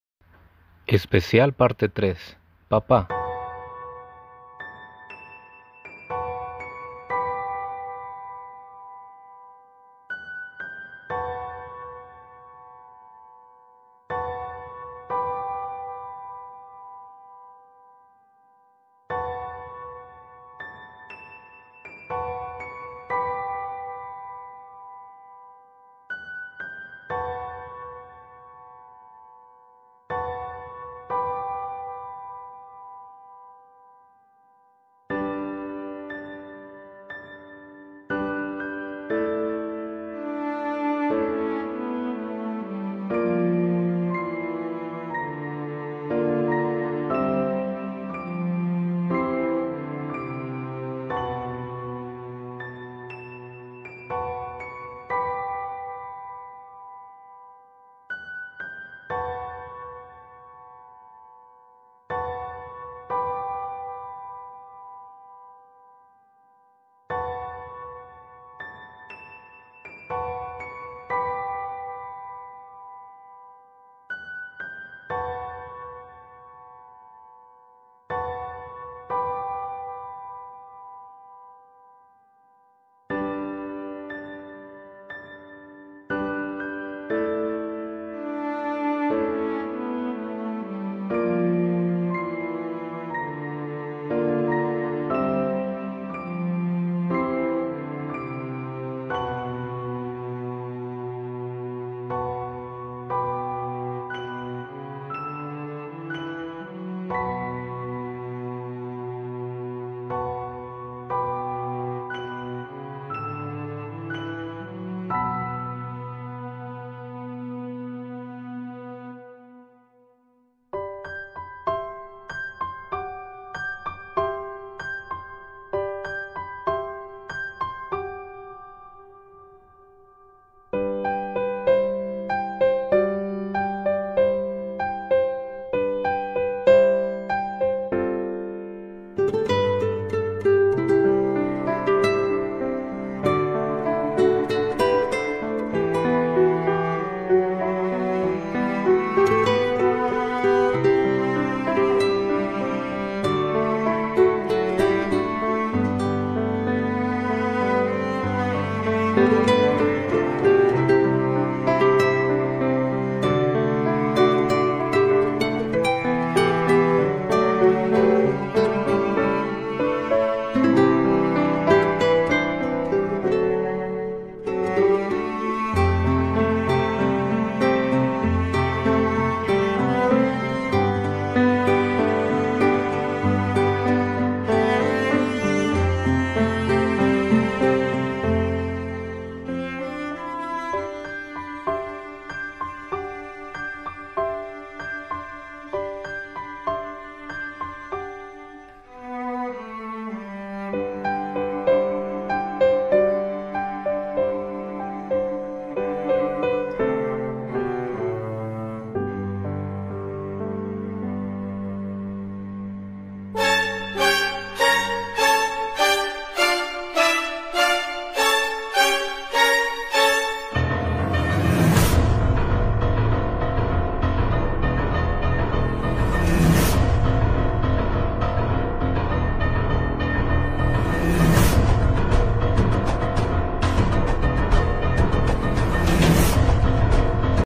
gohan del futuro y goku se reencuentran parte 3
finalmente a llegado el capítulo más emocionante. llegó el momento chicos.